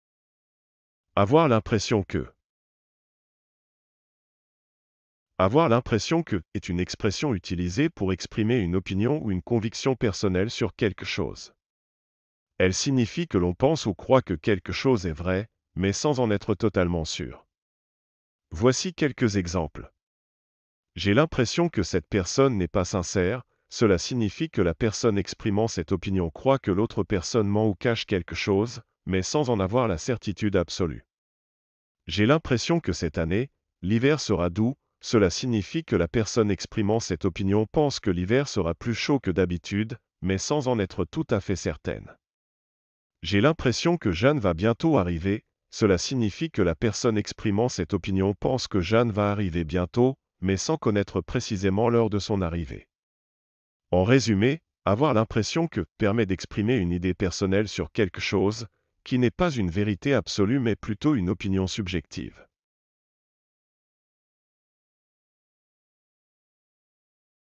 Avoir l'impression que
"Avoir l'impression que" est une expression utilisée pour exprimer une opinion ou une conviction personnelle sur quelque chose. Elle signifie que l'on pense ou croit que quelque chose est vrai, mais sans en être totalement sûr.
Voici quelques exemples :
- J'ai l'impression que cette personne n'est pas sincère : cela signifie que la personne exprimant cette opinion croit que l'autre personne ment ou cache quelque chose, mais sans en avoir la certitude absolue.
- J'ai l'impression que cette année, l'hiver sera doux : cela signifie que la personne exprimant cette opinion pense que l'hiver sera plus chaud que d'habitude, mais sans en être tout à fait certaine.
- J'ai l'impression que Jeanne va bientôt arriver : cela signifie que la personne exprimant cette opinion pense que Jeanne va arriver bientôt, mais sans connaître précisément l'heure de son arrivée.
En résumé, "avoir l'impression que" permet d'exprimer une idée personnelle sur quelque chose, qui n'est pas une vérité absolue mais plutôt une opinion subjective.